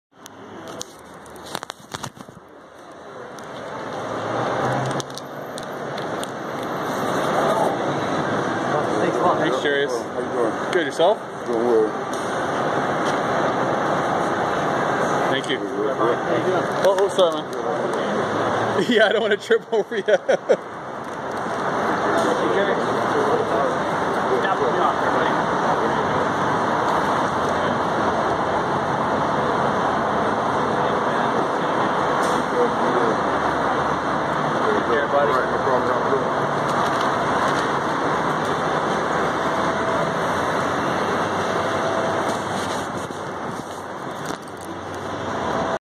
Jarious Jackson of the Saskatchewan Roughriders signs autographs for fans in Edmonton
Saskatchewan Roughriders Quarterback coach Jarious Jackson signs autographs for fans at team hotel in Edmonton